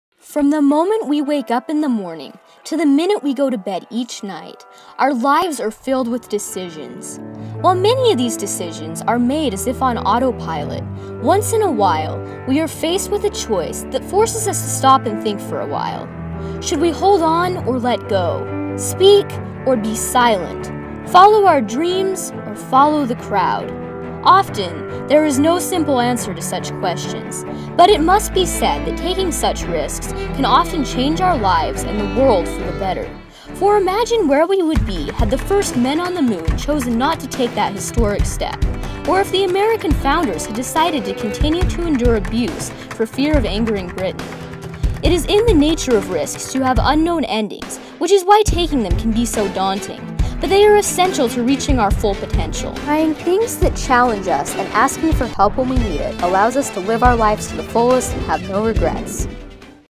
Risks Worth Taking
High School PSA Nominee
10th Anniversary Canyons District Film Festival
by Emily Erickson - Park Lane Elementary